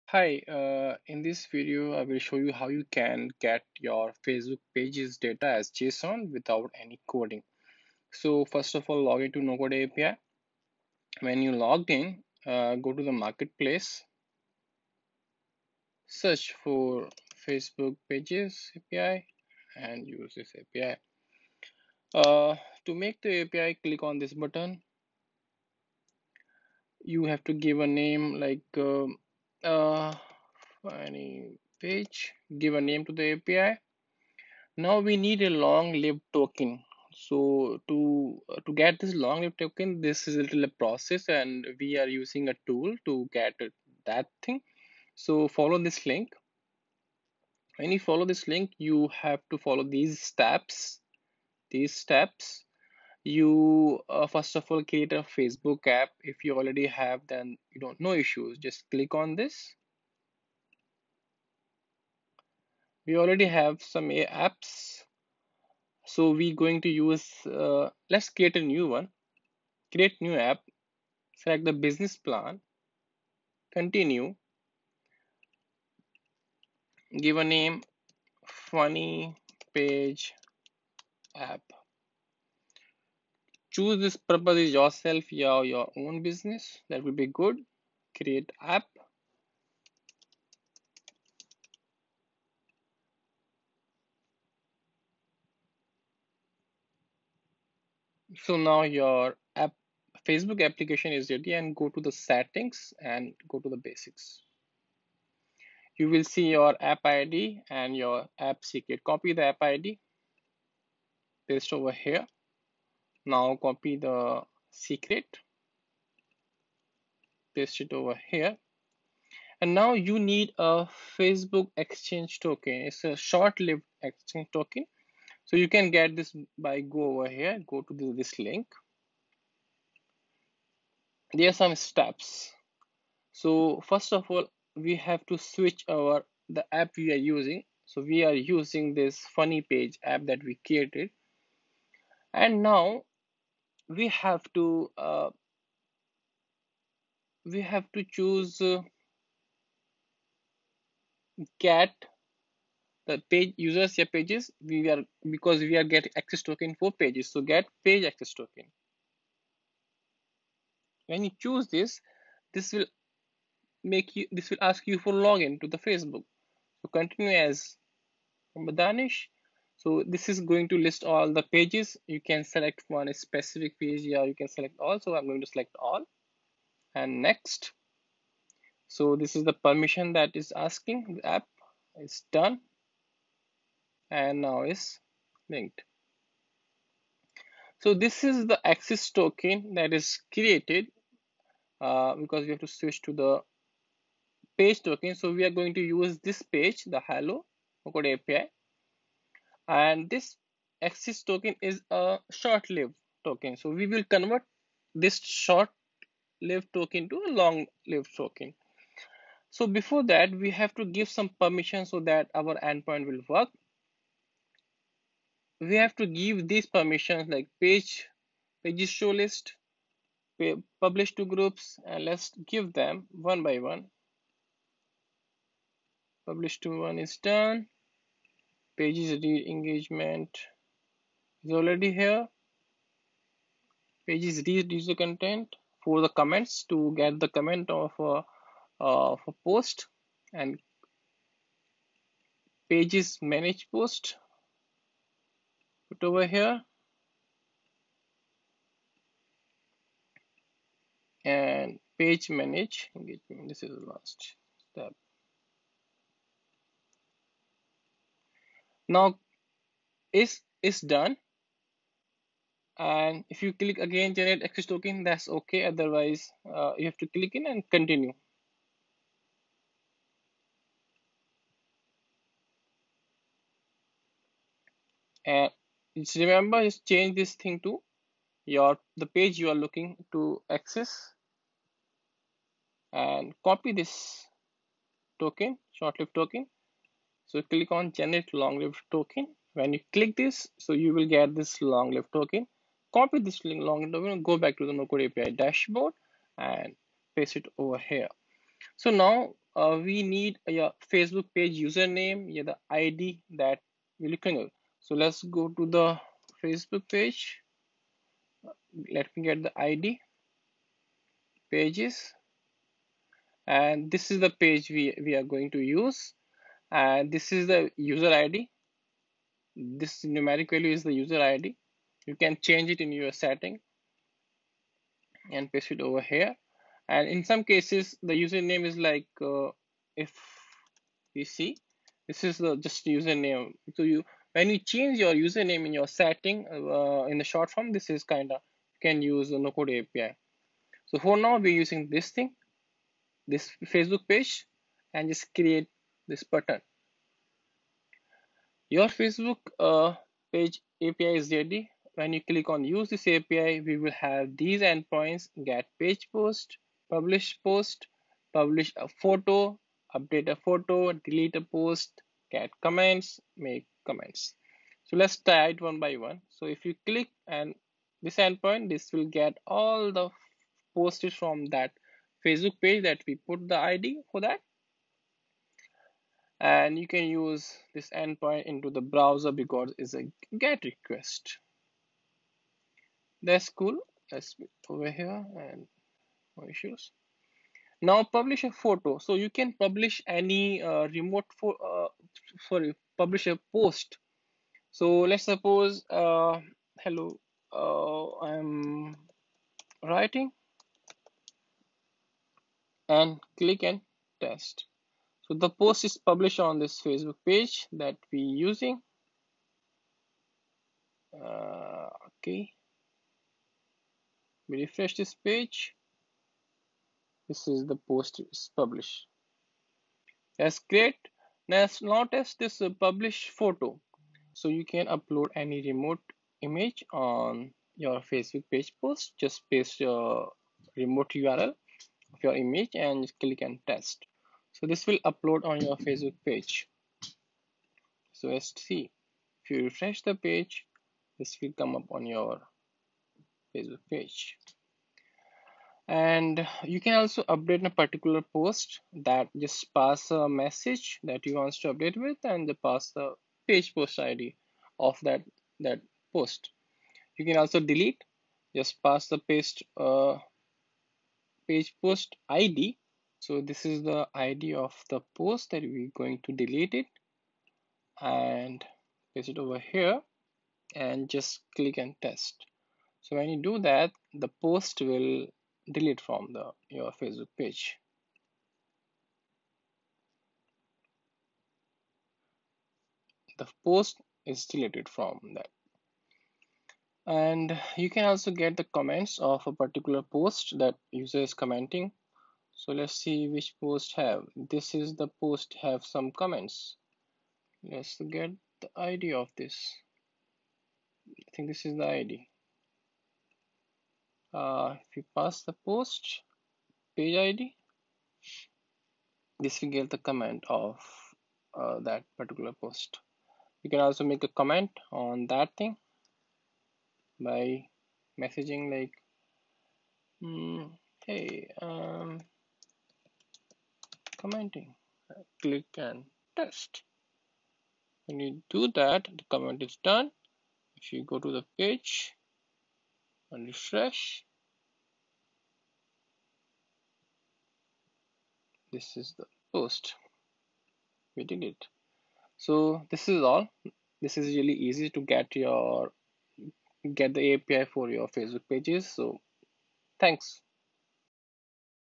Facebook Pages API without code — NoCodeAPI
Get Facebook Pages API Long Live Token and get Facebook page posts as JSON.

Get Page Posts - Retrieve your Facebook page post data as JSON.
Publish Post - Publish a post on the Facebook page with a simple API.
Publish Photo - Publish a photo on your Facebook page via a photo remote URL.
Update Post - You can update any post with a page post id.
Delete Post - You can delete Facebook page posts without any coding
Get Comments - List all the comments of a Facebook page post
Make Comment - Write a comment on a Facebook page post.